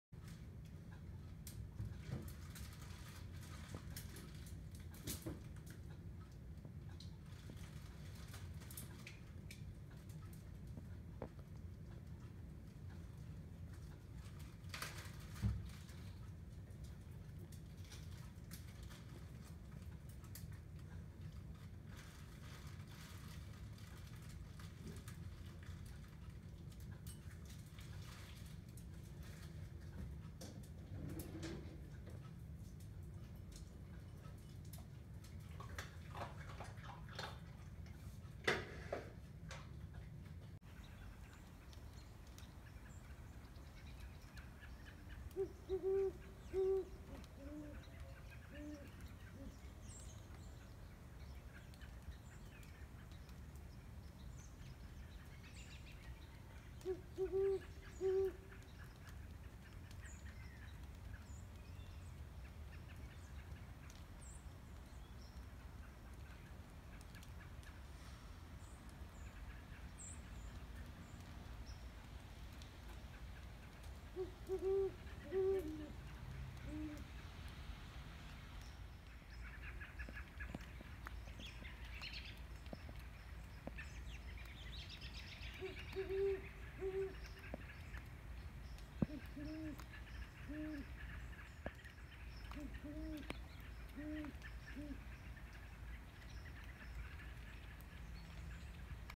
2017.12 Birds
Downy Woodpecker and Great Horned Owls right out my backyard in Lexington, KY